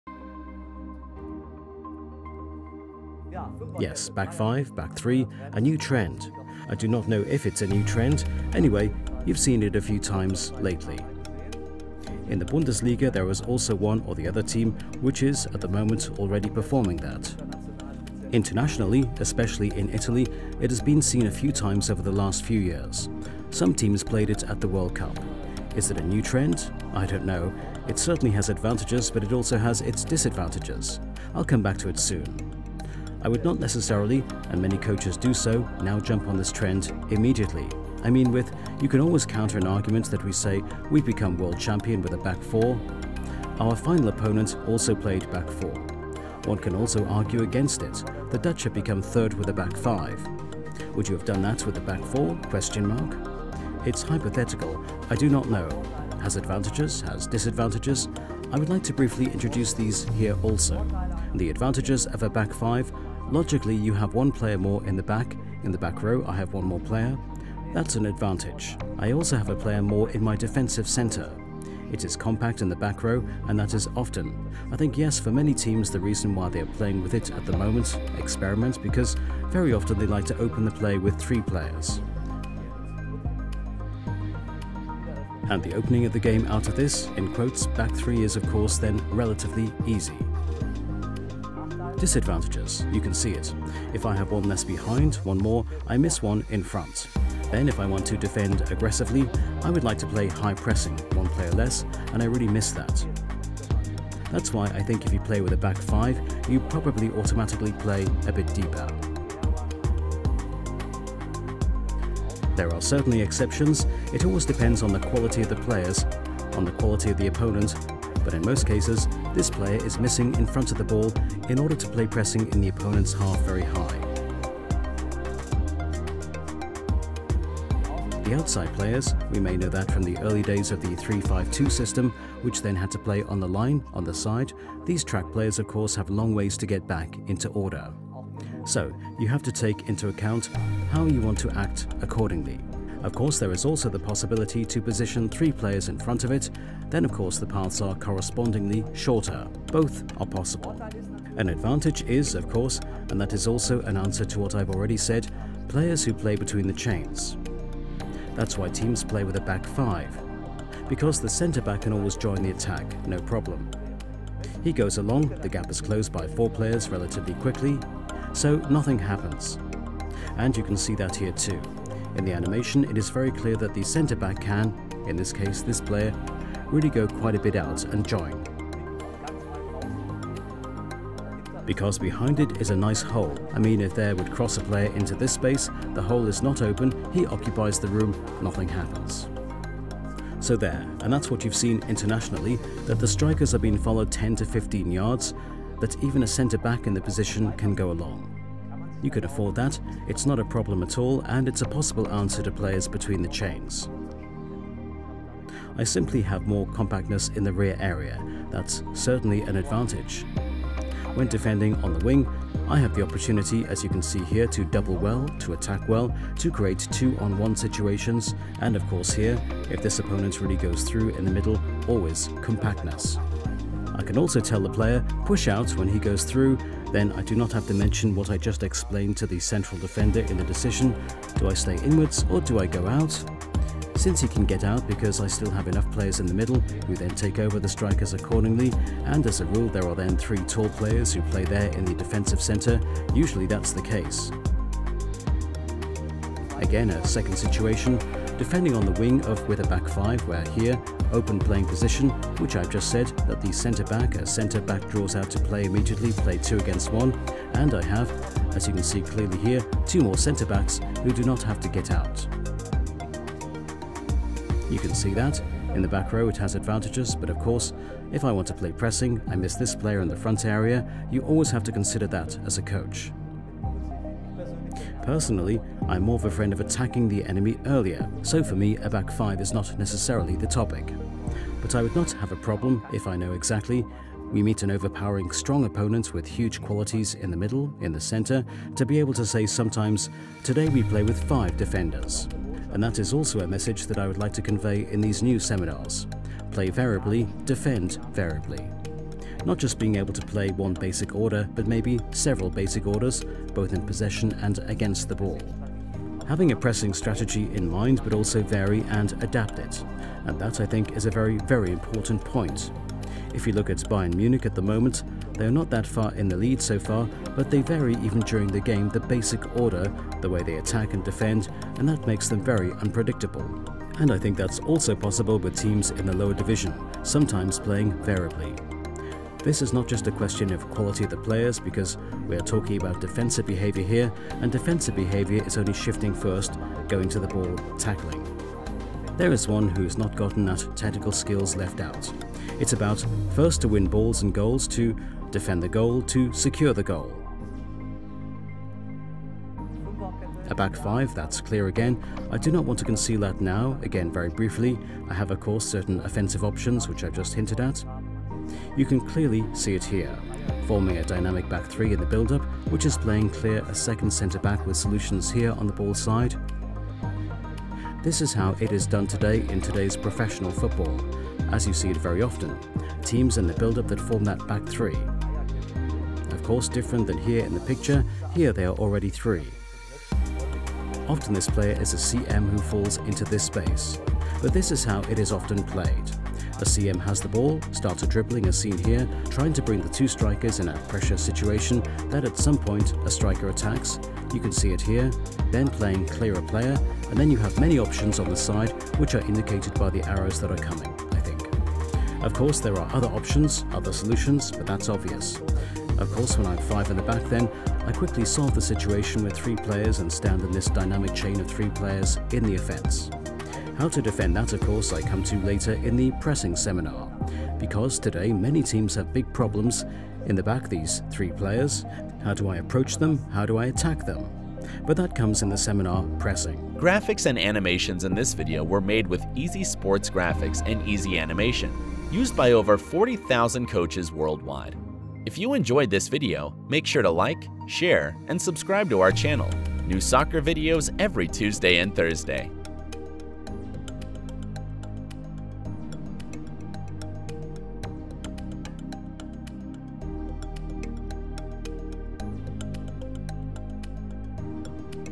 Soccer Trends in Defending - Defending with Back Five
Ralf Peter, coaches instructor for the German Football Association (DFB) and youth soccer expert of worldwide recognition, has coached many German youth national teams and is a regular speaker on coaches conferences worldwide.

In this video he covers defending with back five. Is it a trend in soccer defending?

This video was taken from his seminar series "Current Defending in Space", which covers topics such as defending deep in the own half, basic movements of the back four
and as shown in this video, back five - back three - a new trend?

THE COMPLETE VIDEO:
The complete video "CURRENT TRENDS OF DEFENDING IN SPACE" is available as digital download and stream here: